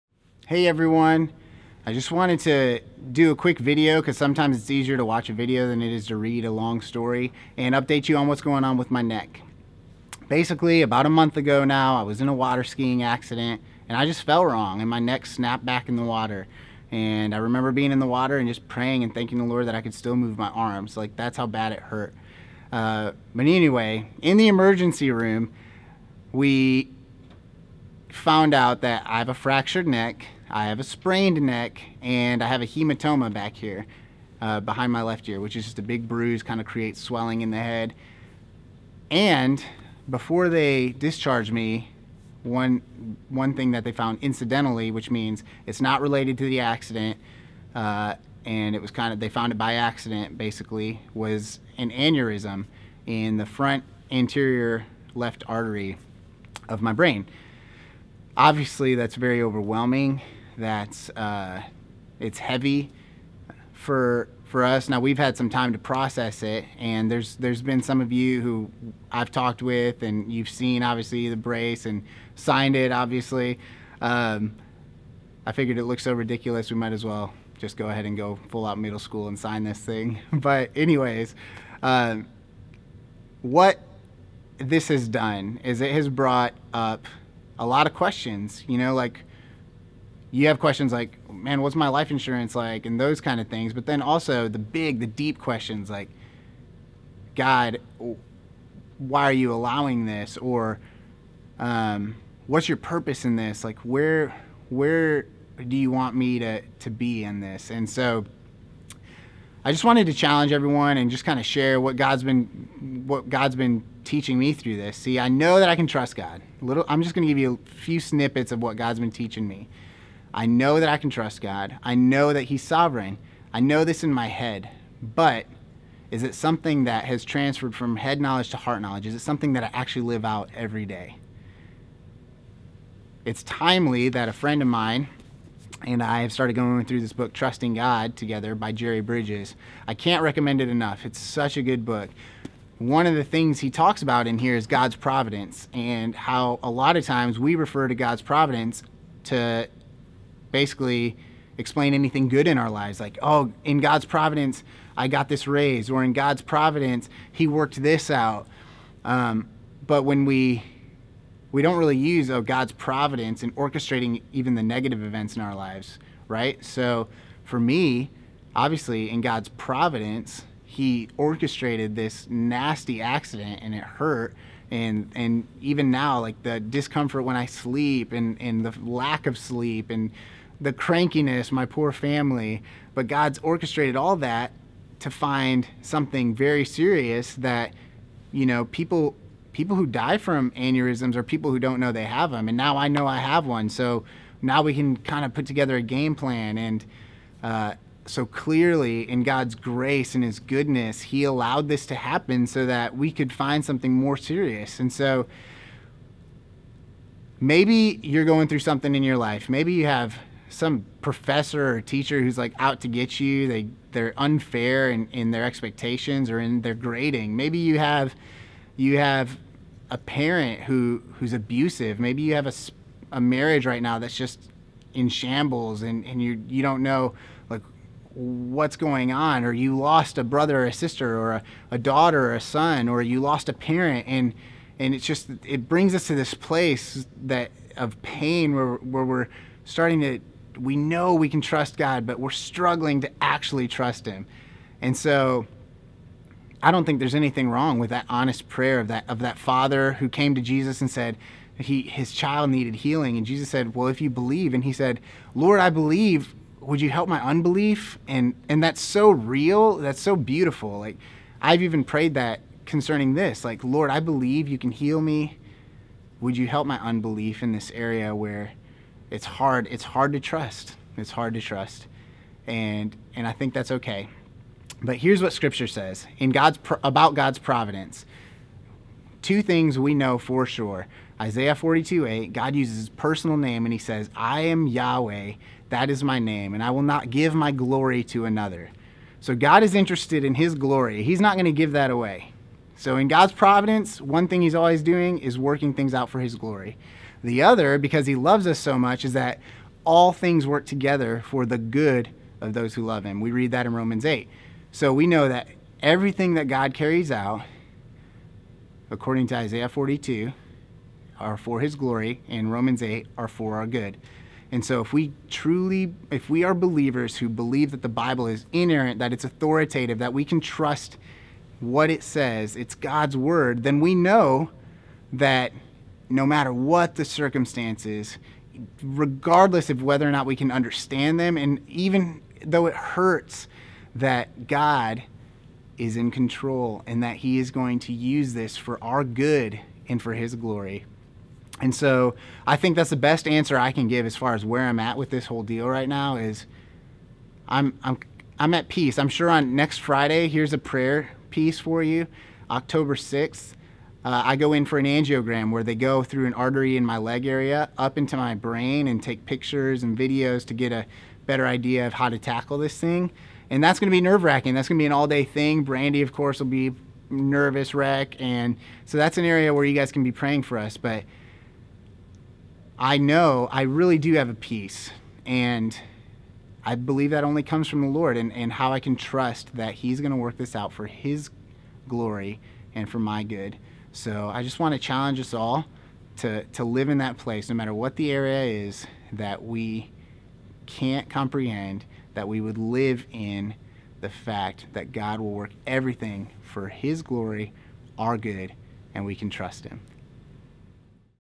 Jordan Santos Fractured Neck & Aneurysm Update
Sometimes we can wonder why God allows certain things to happen. But if we read Scripture, we can know for sure that according to Isaiah 42:8 God's purposes will always be for His own glory. It doesn't stop there though. According to Romans 8:28 all things work together for the good of those who love Him. So as believers, we should take great comfort in this. No matter what life's circumstances, we can always trust that God will work all things together for His glory and our good.